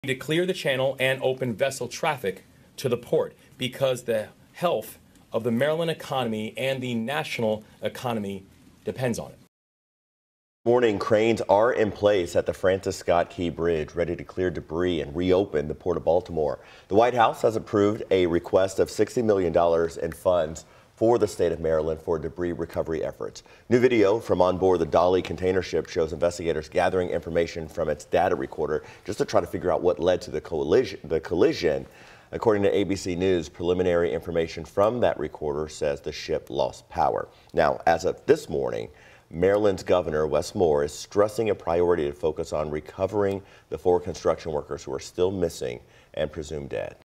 Baltimore bridge collapse update | Efforts underway to reopen port
According to ABC News, preliminary information from the recorder says the ship lost power prior to crashing into the bridge.